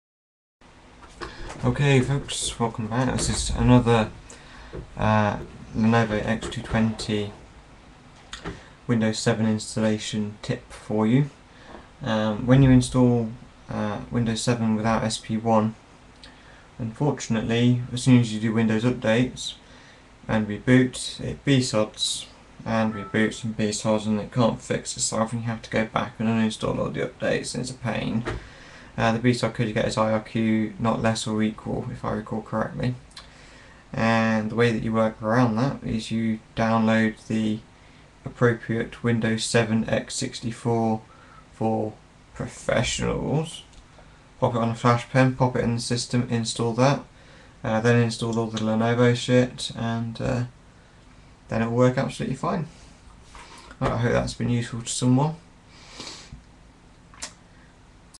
Lenovo x220 Windows 7 Fixing BSOD after initial updates irq not less or equal
After getting over the hurdles of installing windows 7 on your thinkpad via usb (see my other video on how to do this) after the first windows updates install you cant boot up anymore.  You get bsod irq not less or equal.

To fix this download the windows 7 sp1 for professionals.  Install this and then continue as normal.